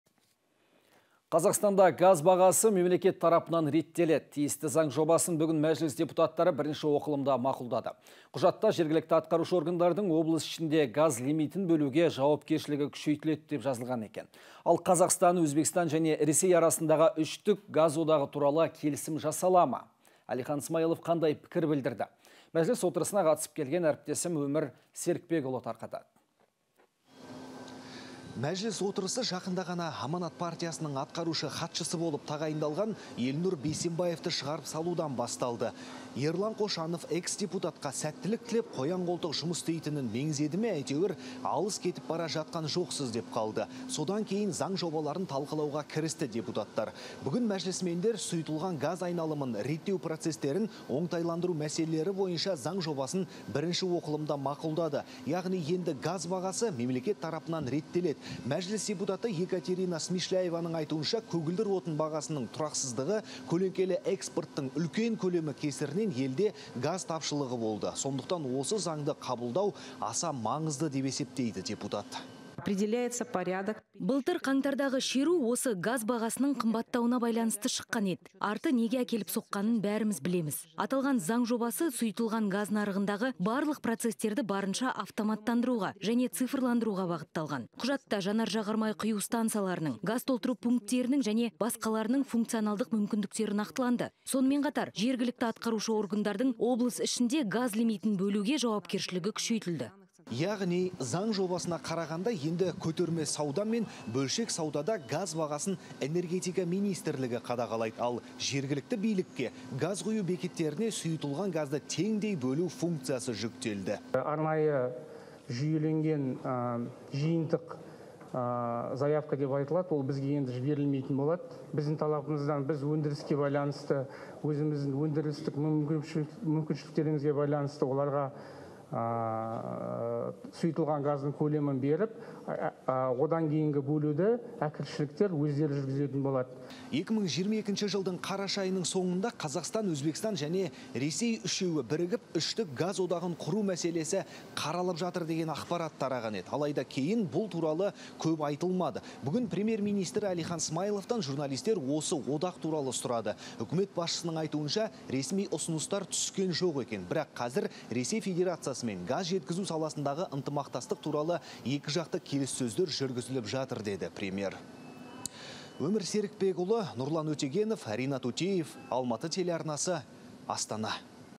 Қазақстанда газ бағасы мемлекет тарапынан реттеледі
Қазақстанда газ бағасы мемлекет тарапынан реттеледі. Тиісті заң жобасын бүгін Мәжіліс депутаттары бірінші оқылымда мақұлдады. Құжатта «жергілікті атқарушы органдардың облыс ішінде газ лимитін бөлуге жауапкершілігі күшейтіледі» деп жазылған, деп хабарлайды Almaty.tv тілшісі.